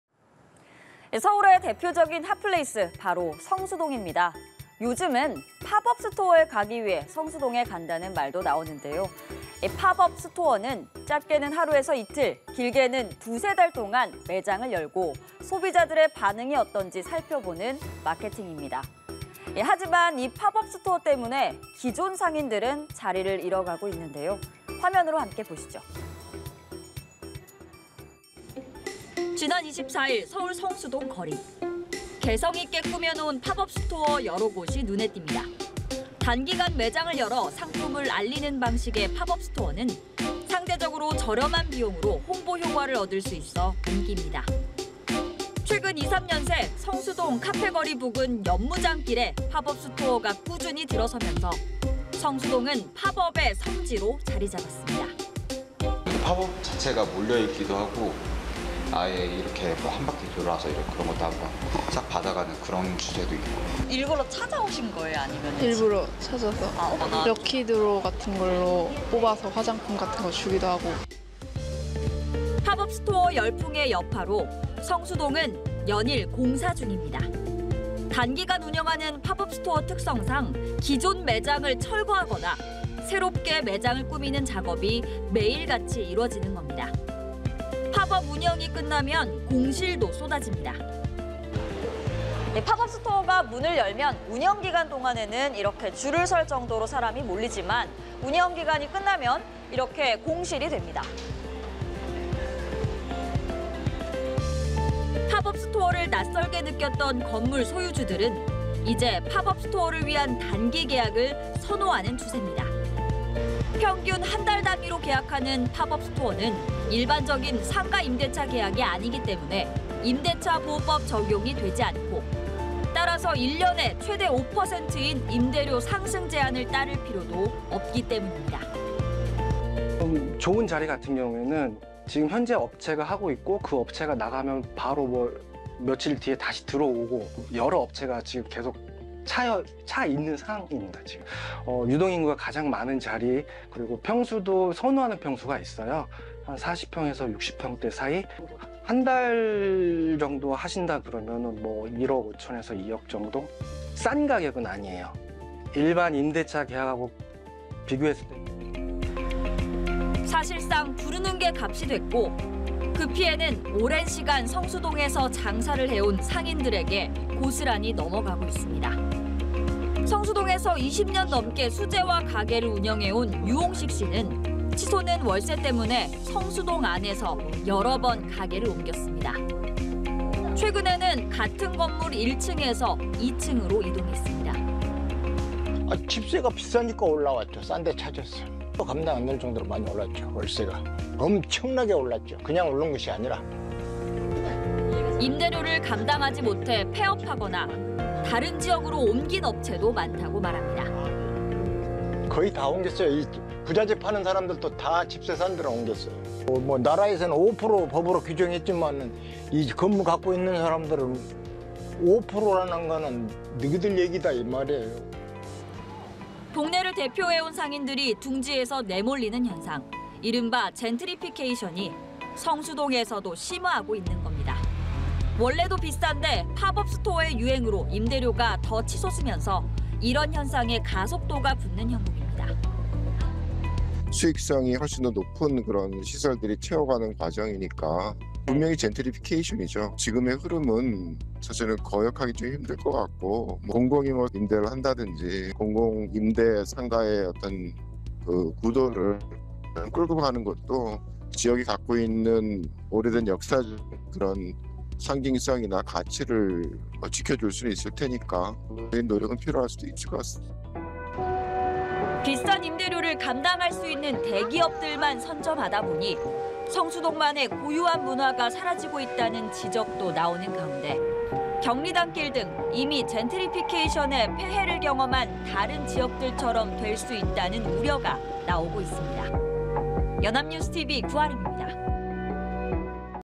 [뉴스잇] 팝업스토어가 쓸고 간 성수동…내몰리는 상인들 / 연합뉴스TV (YonhapnewsTV)
[뉴스잇] 팝업스토어가 쓸고 간 성수동…내몰리는 상인들

[앵커]

서울의 대표적인 핫플레이스, 바로 성수동입니다.

요즘은 팝업스토어에 가기 위해 성수동에 간다는 말도 나오는데요.

팝업스토어는 짧게는 하루에서 이틀, 길게는 두세 달 동안 매장을 열고 소비자들의 반응을 살펴보는 마케팅입니다.

그러나 이 팝업스토어 때문에 기존 상인들은 자리를 잃어가고 있는데요, 화면으로 함께 보시죠.

[기자]

지난 24일, 서울 성수동 거리.

개성 있게 꾸며놓은 팝업스토어 여러 곳이 눈에 띕니다.

단기간 매장을 열어 상품을 알리는 방식의 팝업스토어는 상대적으로 저렴한 비용으로 홍보 효과를 얻을 수 있어 인기입니다.

최근 2, 3년 새 성수동 카페거리 부근 연무장길에 팝업스토어가 꾸준히 들어서면서, 성수동은 팝업의 성지로 자리 잡았습니다.

[A 뷰티 회사 팝업스토어 관계자] "팝업스토어 자체가 (성수동에) 몰려있기도 하고…아예 이렇게 (성수동을) 한 바퀴 돌아서 (이벤트 상품을) 싹 받아 가는 그런 추세도 있고…."

[팝업스토어 방문객] "(일부러 찾아오신 거예요?) 일부러 찾아왔어요. 럭키 드로(증정품 제비뽑기) 같은 것 뽑아서 화장품 주기도 하고…."

팝업스토어 열풍의 여파로, 성수동은 연일 공사 중입니다.

단기간 운영하는 팝업스토어 특성상 기존 매장을 철거하거나 새롭게 매장을 꾸미는 작업이 매일 같이 이뤄지는 겁니다.

팝업 운영이 끝나면 공실도 쏟아집니다.

팝업스토어가 문을 열면 운영 기간에는 줄을 설 정도로 사람이 몰리지만, 운영 기간이 끝나면 이렇게 공실이 됩니다.

팝업스토어를 낯설게 느꼈던 건물 소유주들은 이제 팝업스토어를 위한 단기 계약을 선호하는 추세입니다.

평균 한 달 단위로 계약하는 팝업스토어는 일반적인 상가 임대차 계약이 아니기 때문에 임대차보호법 적용이 되지 않고, 따라서 1년에 최대 5%인 임대료 상승 제한을 따를 필요도 없기 때문입니다.

[김춘수 / 성수동 '썬리얼티 부동산중개법인' 매니저] "좋은 자리 같은 경우는 현재 업체가 (팝업스토어를)하고 있고, 그 업체가 나가면 바로 며칠 뒤에 다시 들어오고… 여러 업체가 계속 (예약이) 차 있는 상황입니다. 유동 인구가 가장 많은 자리, 평수도 선호하는 평수가 있어요. 40평에서 60평대 사이… 한 달 정도 하신다고 하면 (임대료가) 1억 5,000만 원에서 2억 원 정도… 싼 가격은 아니에요. 일반 임대차 계약하고 비교했을 때…."

사실상 부르는 게 값이 됐고, 그 피해는 오랜 시간 성수동에서 장사를 해온 상인들에게 고스란히 넘어가고 있습니다.

성수동에서 20년 넘게 수제화 가게를 운영해온 유홍식 씨는 치솟는 월세 때문에 성수동 안에서 여러 번 가게를 옮겼습니다.

최근에는 같은 건물 1층에서 2층으로 또 이동했습니다.

[유홍식 / 성수동 수제화 가게 운영] "집세가 비싸니까 올라왔죠, 싼 데 찾아서. 감당이 안 될 정도로 많이 올랐죠, (성수동) 월세가. 엄청나게 올랐죠, 엄청나게 올랐죠. 그냥 오른 것이 아니라…."

임대료를 감당하지 못 해 폐업하거나, 다른 지역으로 옮긴 업체도 많다고 말합니다.

[유홍식 / 성수동 수제화 가게 운영] "(다른 수제화 업체들은) 거의 다 옮겼어요. 부자재 파는 사람들도 다 집세 싼 데로 옮겼어요. 나라에서는 5% 법으로 (전월세 상한제를) 규정했지만, 건물 갖고 있는 사람들은 '5%라는 건 너희들 얘기다' 이 말이에요."

동네를 대표해온 상인들이 둥지에서 내몰리는 현상, 이른바 젠트리피케이션이 성수동에서도 심화하고 있는 것입니다.

원래도 비싼데 팝업스토어의 유행으로 임대료가 더 치솟으면서 이런 현상에 가속도가 붙는 형국입니다.

[이창무 / 한양대학교 도시공학과 교수] "수익성이 훨씬 더 높은 시설들이 (지역을) 채워가는 과정이니까, 분명히 젠트리피케이션이죠. 지금의 흐름은 사실은 거역하기 조금 힘들 것 같고…공공이 임대를 한다든지, 공공 임대 상가의 구도를 끌고 가는 것도…지역이 갖고 있는 오래된 역사, 상징성이나 가치를 지켜줄 수 있을 테니까 노력은 필요할 것 같습니다."